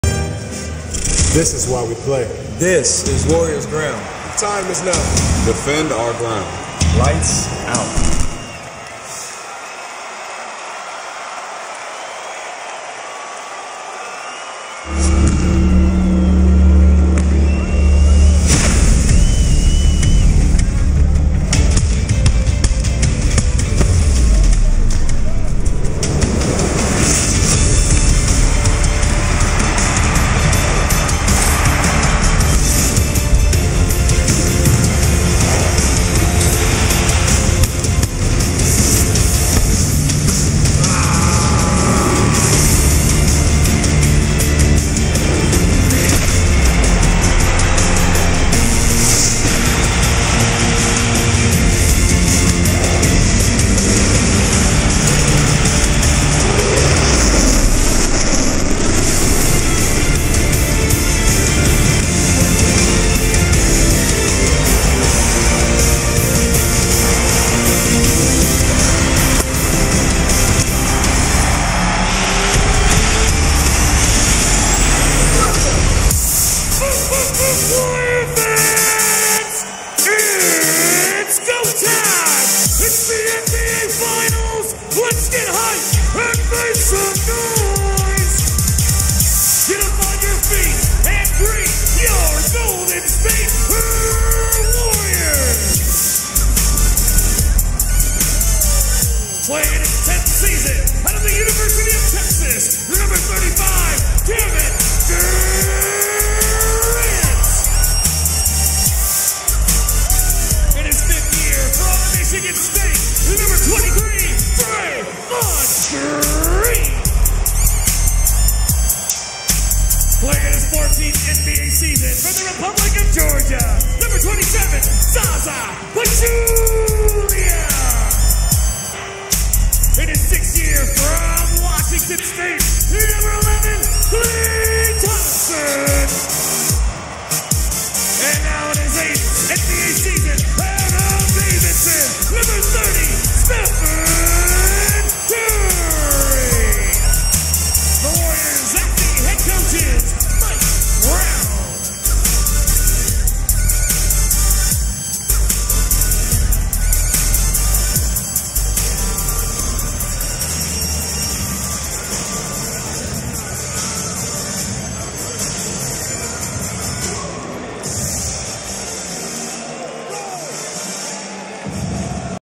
Golden State Warriors players intro NBA Finals 2017
Game 1 of 2017 NBA Finals.
Oracle Arena, Oakland, CA. Jun 1, 2017.